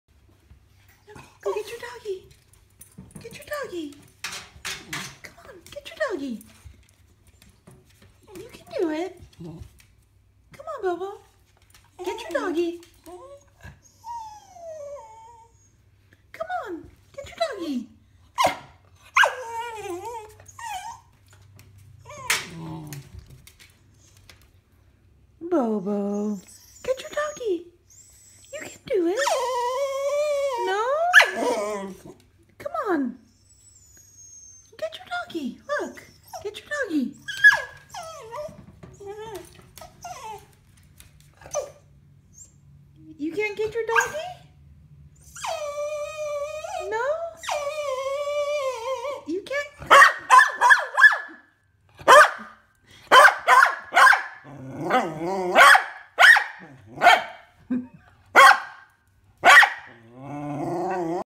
Bobo's Toy
Bobo's stuffed animal is trapped in a giant tin tub, but he's too scared to get it out